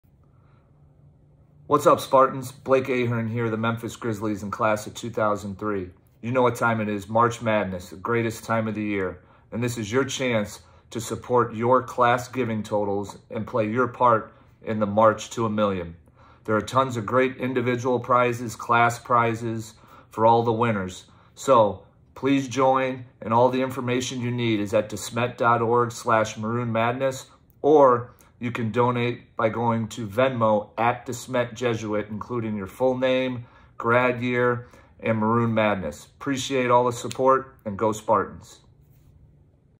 Maroon Madness 2023 - Blake Ahearn
De Smet Jesuit Alum and current assistant coach for the Memphis Grizzlies, Blake Ahearn, challenging all our Spartan Alums to step up in the March to a Million as we kick off Maroon Madness.  Let's Go, Spartan Country!!